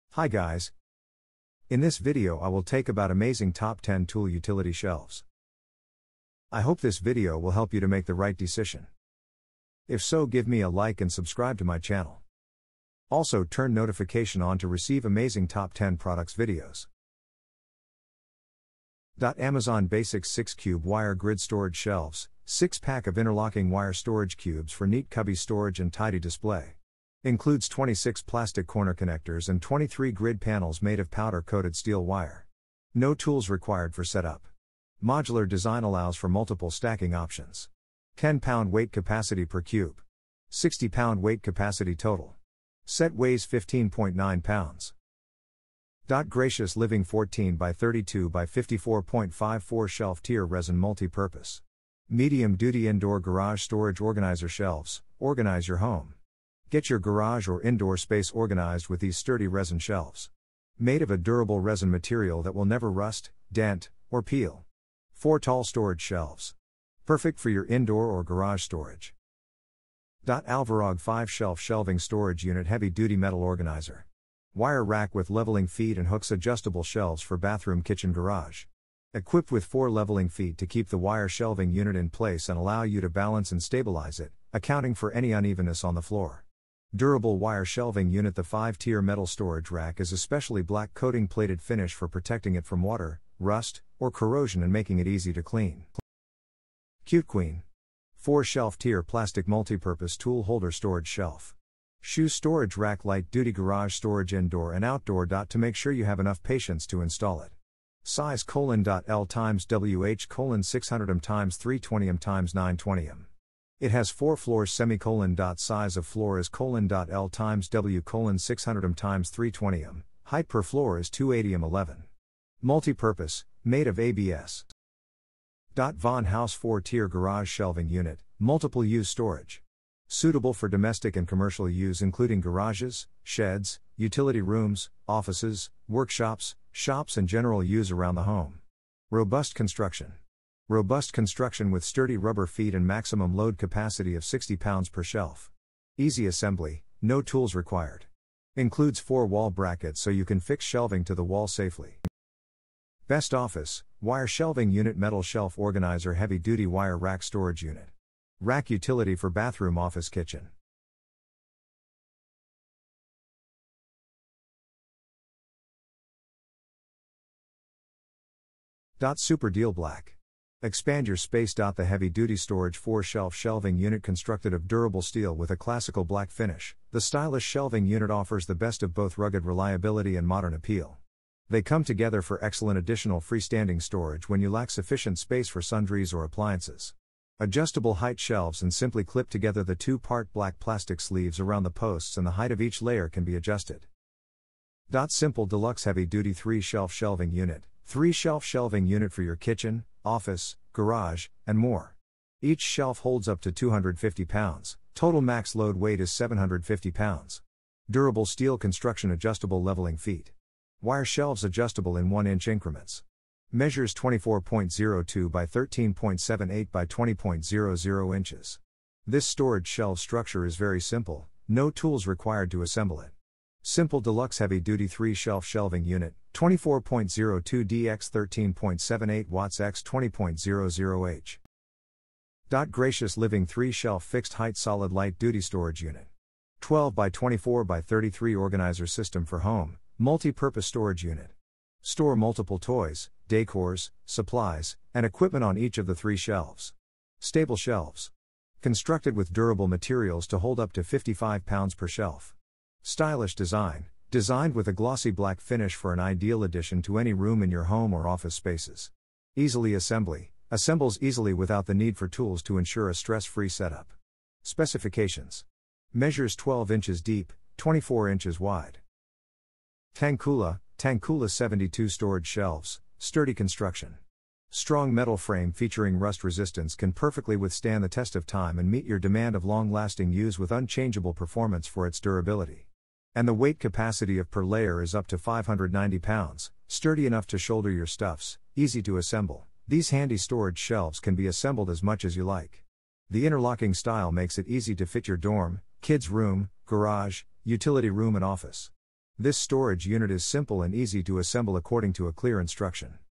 Top 10 Tool Utility Shelves You Can Buy November 2022
Amazon Basics 6-Cube Wire Grid Storage Shelves

Gracious Living 14  x 32  x 54.5  4-Shelf Tier Resin Multi-Purpose Medium Duty Indoor Garage Storage Organizer Shelves

ALVOROG 5-Shelf Shelving Storage Unit Heavy Duty Metal Organizer Wire Rack with Leveling Feet and Hooks Adjustable Shelves for Bathroom Kitchen Garage  23.2Lx13.4Wx59.1H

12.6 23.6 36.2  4-Shelf Tier Plastic Multi-Purpose Tool Holder Storage Shelf Shoe Storage Rack Light Duty Garage Storage Indoor and Outdoo

VonHaus 4 Tier Garage Shelving Unit  Pack of 2  - Black Plastic Interlocking Utility Storage Shelves - Each Unit: 52 x 24 x 12 inche

Wire Shelving Unit Metal Shelf Organizer Heavy Duty Wire Rack Storage Unit Rack Utility for Bathroom Office Kitchen  Black

SUPER DEAL Black 4-Shelf Heavy Duty Storage Wire Shelving Unit for Restaurant Garage Pantry Kitchen Garage Rack  36L x 14W x 54H

Gracious Living 3 Shelf Fixed Height Solid Light Duty Storage Unit 12 x 24 x 33 Organizer System for Home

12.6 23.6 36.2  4-Shelf Tier Plastic Multi-Purpose Tool Holder Storage Shelf Shoe Storage Rack Light Duty Garage Storage Indoor and Outdoo

Simple Deluxe Heavy Duty 3-Shelf Shelving Unit

Tangkula 72  Storage Shelves

 To Get it you should Pay :: $199.9